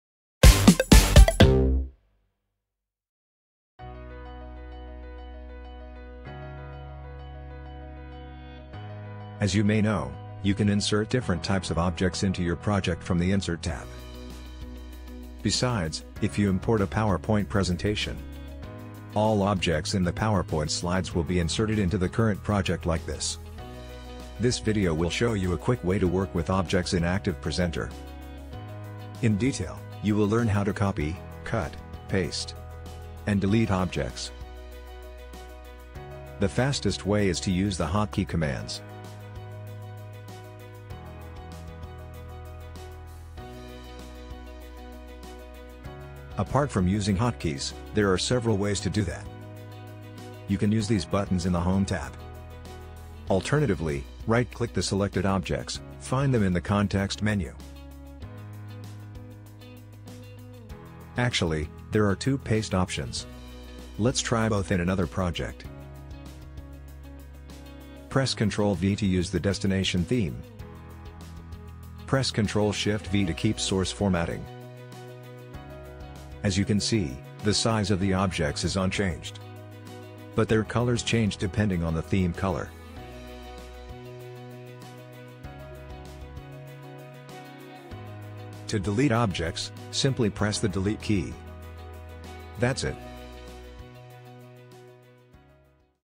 How to Copy, Cut, Paste and Delete Objects - ActivePresenter 8
ActivePresenter lets you Copy, Cut, Paste and Delete objects quickly. Let’s learn how to use these commands for objects in this video.

In this video:
0:43 - Copy, Cut, Paste Objects
1:24 - Delete Objects